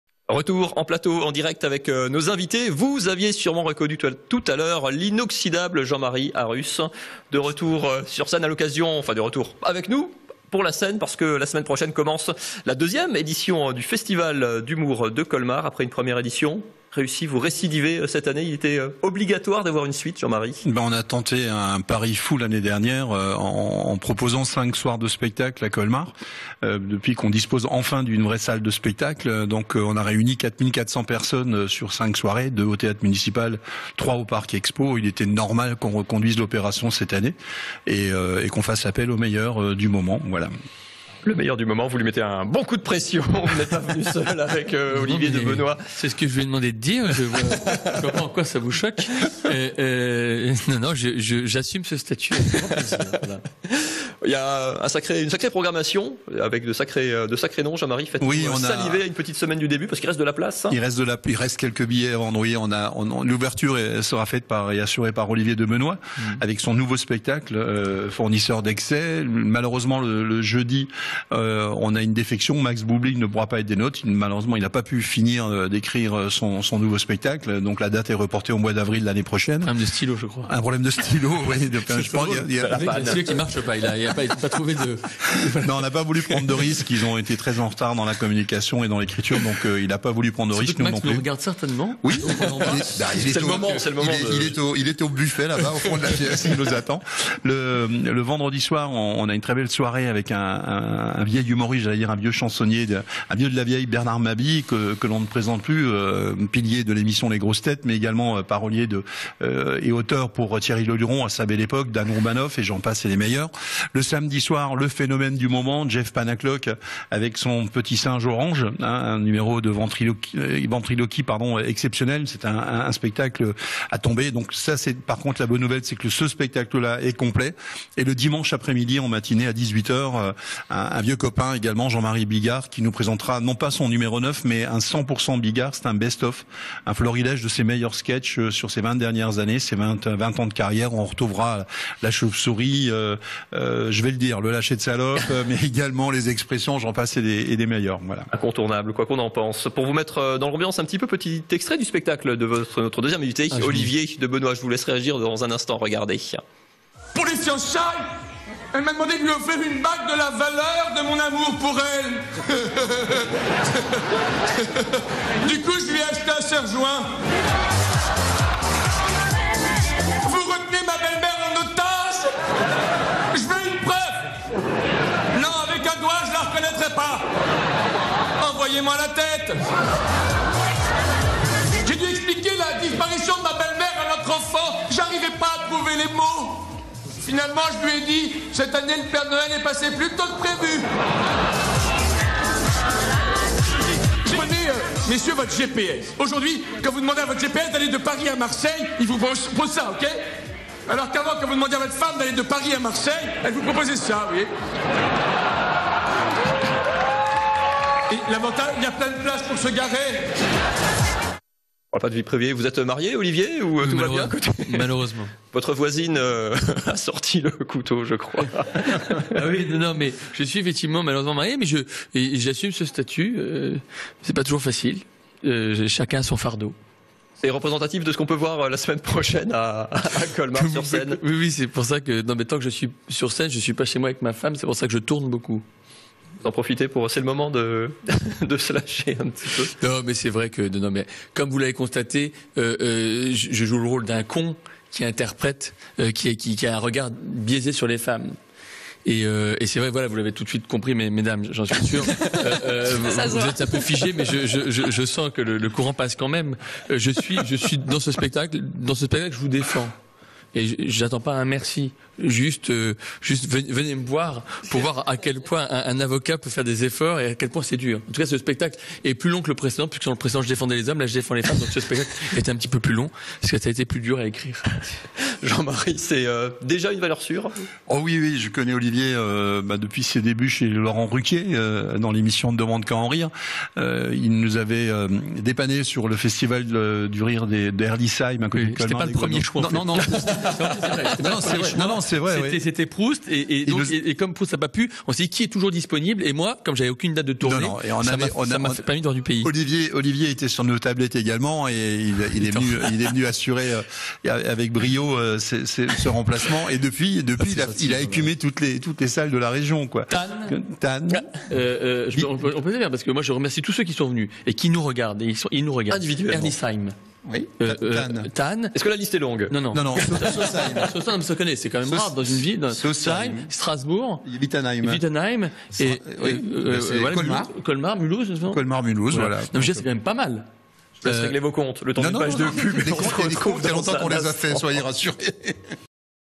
ODB tête d'affiche du festival d'humour de Colmar
Sites webs/blogs, copiez cette vidéo avec le bouton "Intégrer" !
 Jean-Marie Arrus, organisateur du festival d'humour de Colmar, nous présente l'édition 2014 accompagné de sa tête d'affiche : Olivier de Benoist !